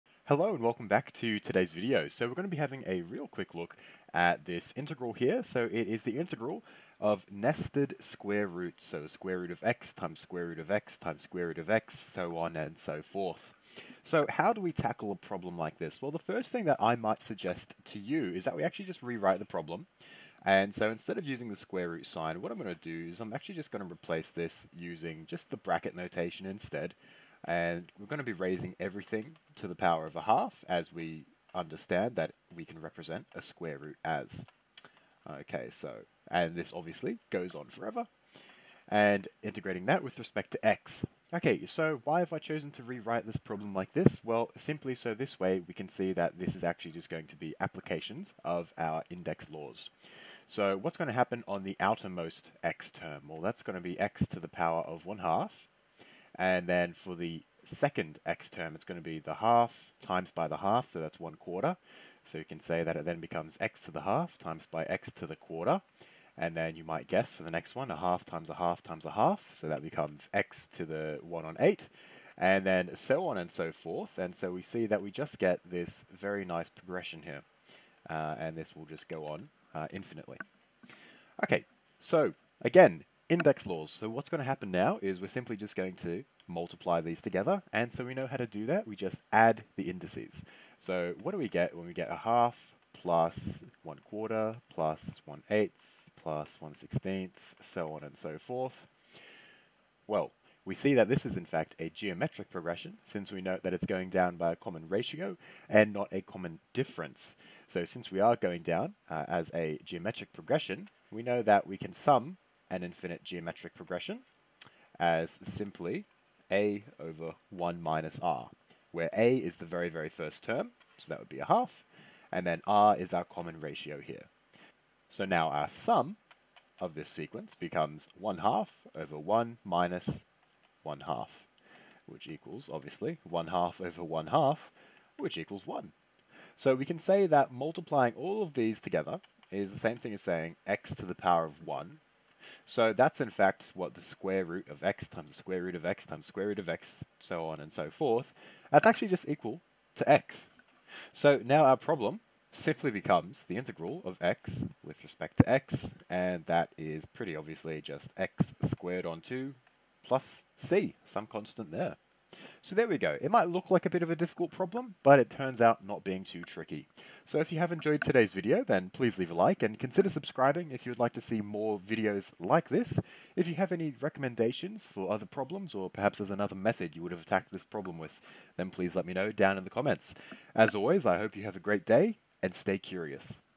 Integrating Infinitely Nested Square Roots
00:00 Intro
00:19 Rewriting the Problem
00:45 Index Laws
01:30 Geometric Progression
02:16 The Simplified Problem
02:30 Final Answer
02:42 Outro